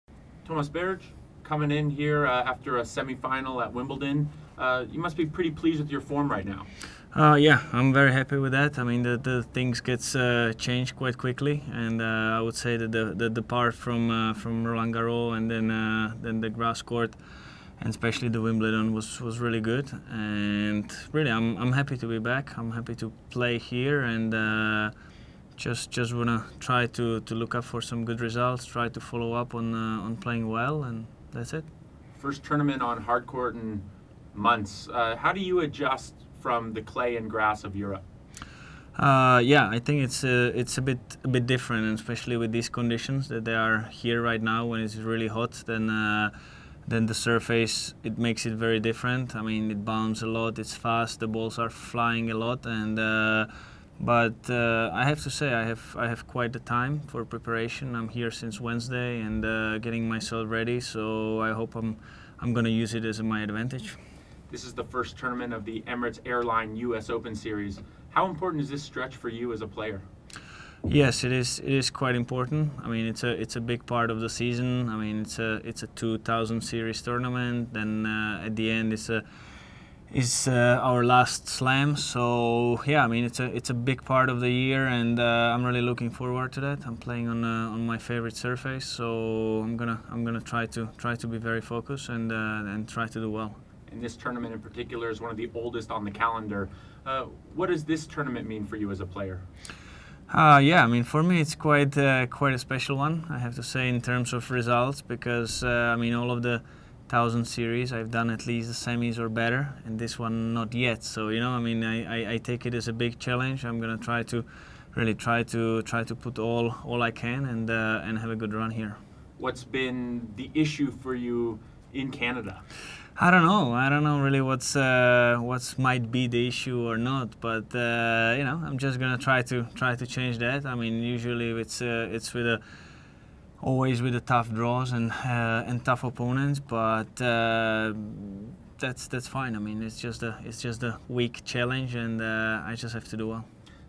Berdych Wants Best Showing Yet In Toronto 2016
Tomas Berdych hopes to post his best results yet at the Rogers Cup in Toronto.